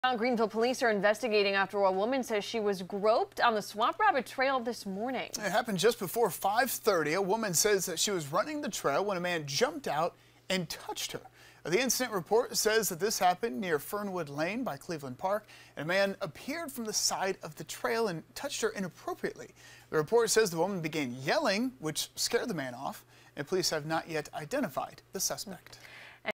Assault on Swamp Rabbit Trail
Greenville Police are investigating after a woman says she was groped on the Swamp Rabbit Trail this morning.